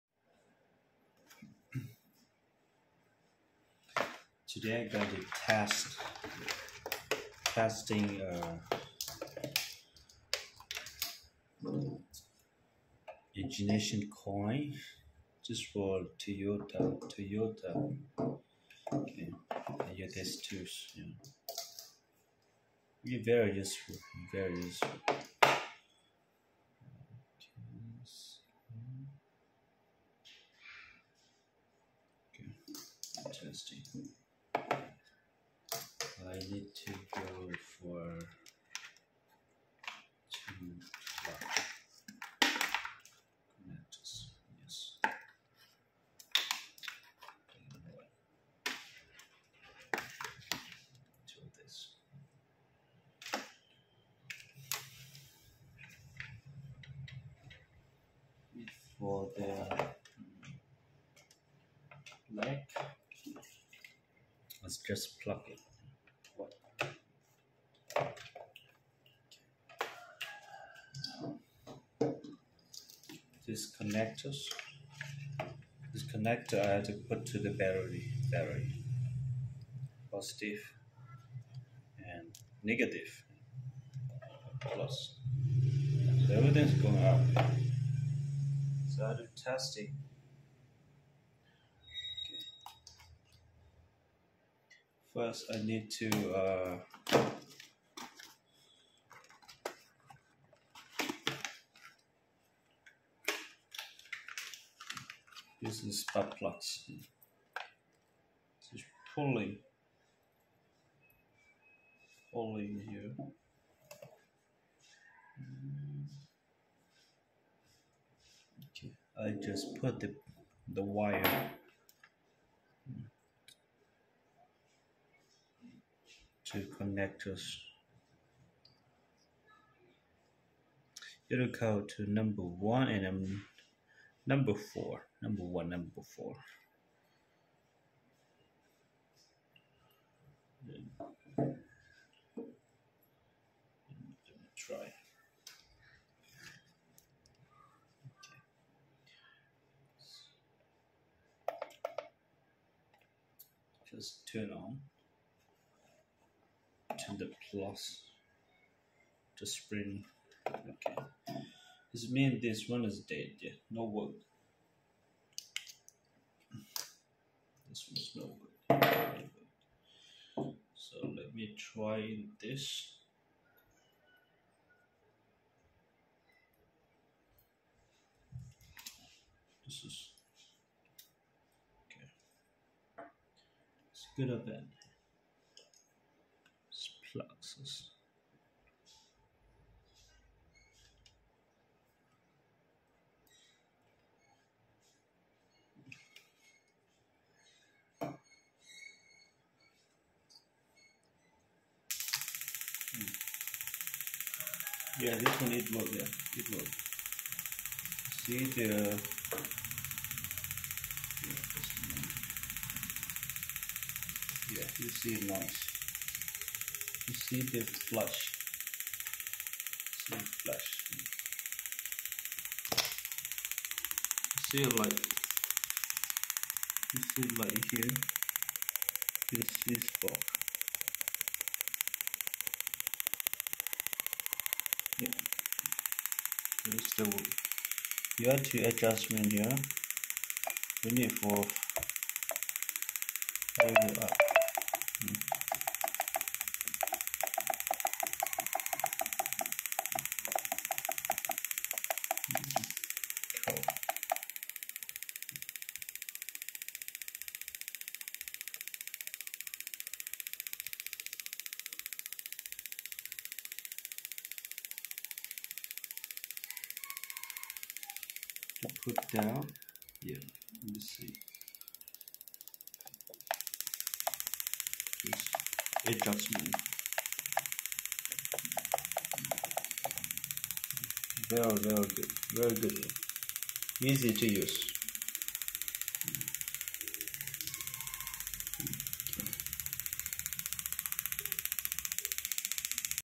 how to check fault ignition coil with auto actuator detector
Offer worldwide shipping:
Connecting the detector to the ignition coil, press the start button of the vehicle, it will indicate whether the ignition coil is working properly or not.

1. Set up the ignition coil to work by connecting it to the actuator detector

2. Switch on the power and connect the actuator detector to the computer.

3. Set up the actuator detector, check the specification for your vehicle and then set up your actuator detector according to those specifications.

4. Check the ignition coil by setting up a test plan, choosing a mode of operation and then carrying out the test on your ignition coils.